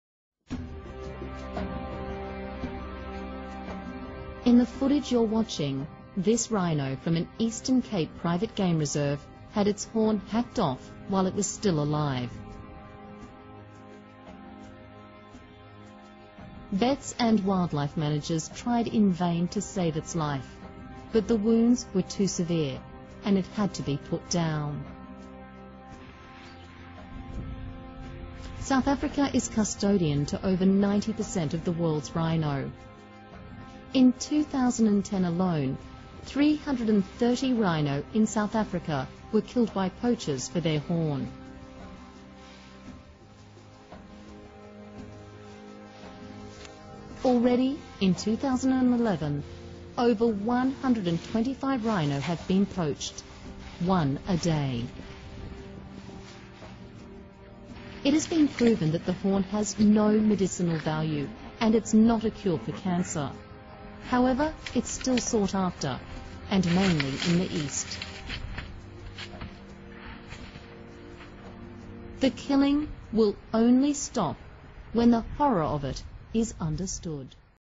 Rinho Horn brutaly removed
This is one of the most horrific forms of animal abuse I've seen to-date! I'm a tolerant man of many things but when it comes to animal abuse I have NONE! We humans evolved to be one of the most intelligent species on the planet. Our intelligences doesn't give us the right to abuse ,exploit or destroy any living creature in this world. Out intelligence should be use to protect all creatures that can not protect themselves and to make this world a better place to live for all. People who's atrocities are like this have no compassion or love for anything in this world accept themselves!